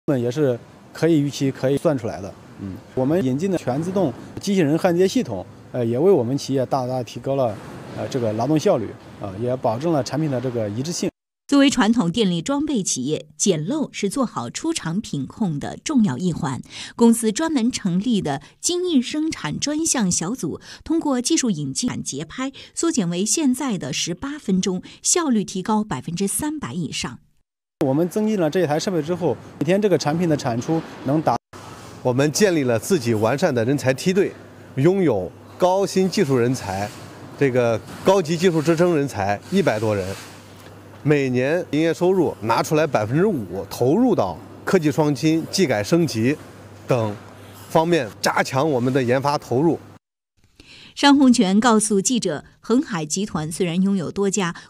Lijian all kinds electric power copper busbar processing machines by the CCTV television interview
Henghai city company, purchasing Lijian all kinds of electric power copper busbar and other processing equipment, the use of the effect is very ideal, by the CCTV television interview.